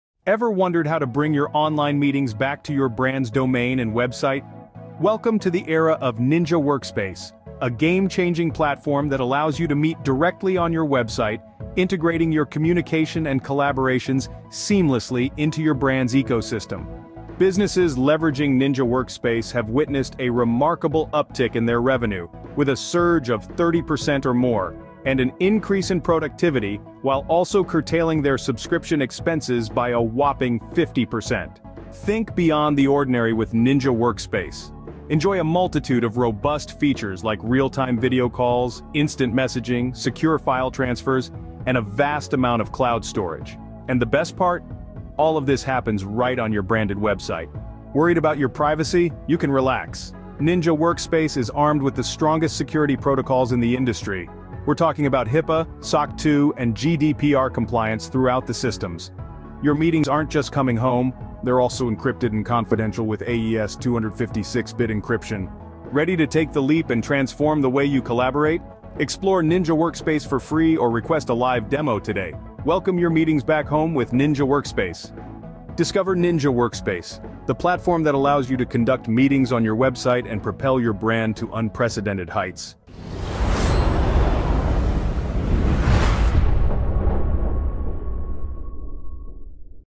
Welcome to NINJA Workspace
🚀 Discover the future of workplace collaboration with NINJA Workspace! In this detailed walkthrough, we'll show you how NINJA Workspace outshines competitors like Zoom, Google Meet, and Microsoft Teams, providing innovative features that streamline your workflow and boost productivity. 🌟

🎯 Whether you're managing a team remotely or coordinating complex projects, NINJA Workspace is designed to make your life easier. Learn how to seamlessly integrate communications, manage tasks, and maintain connectivity, all in one platform.

💬 Drop your questions or feedback in the comments below—we love hearing from you!

🌐 Ready to take your productivity to the next level? Sign up for a FREE NINJA Workspace account today, or request a personalized demo to see firsthand how our tools can transform your workflow. Elevate your team's efficiency with just a few clicks!

Join the revolution in workplace collaboration—because when it comes to productivity, every second counts!